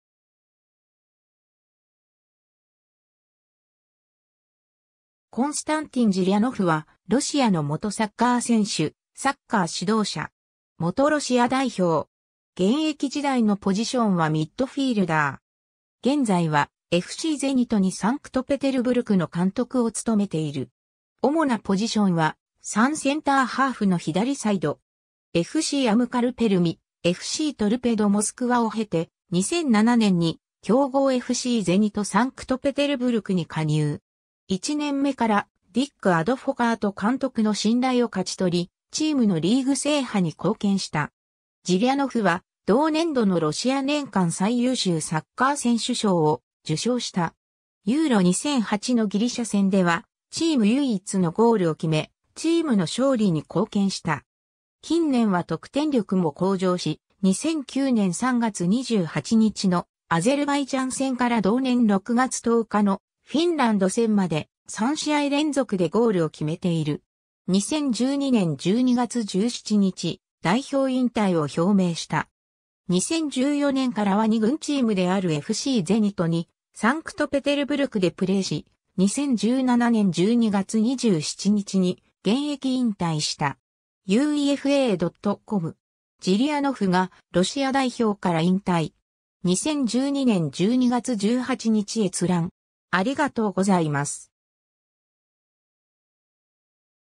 コンスタンティン・ジリャノフ
トルペド・モスクワの選手
ゼニト・サンクトペテルブルクの選手
コンスタンティン・ジリャノフ（Konstantin Zyrianov, 1977年10月5日 - ）は、ロシアの元サッカー選手、サッカー指導者。
元ロシア代表。
現在はFCゼニト2 サンクトペテルブルクの監督を務めている。
 主なポジションは、3センターハーフの左サイド。
FCアムカル・ペルミ、FCトルペド・モスクワを経て、2007年に強豪FCゼニト・サンクトペテルブルクに加入。
1年目から、ディック・アドフォカート監督の信頼を勝ち取り、チームのリーグ制覇に貢献した。
ジリャノフは、同年度のロシア年間最優秀サッカー選手賞を受賞した。
EURO2008のギリシャ戦では、チーム唯一のゴールを決め、チームの勝利に貢献した。
近年は得点力も向上し、2009年3月28日のアゼルバイジャン戦から同年6月10日のフィンランド戦まで、3試合連続でゴールを決めている。
 2012年12月17日、代表引退を表明した。
 2014年からは二軍チームであるFCゼニト2 サンクトペテルブルクでプレーし、2017年12月27日に現役引退した。
 ^ UEFA.com (2012年12月17日). “ジリアノフがロシア代表から引退”. 2012年12月18日閲覧。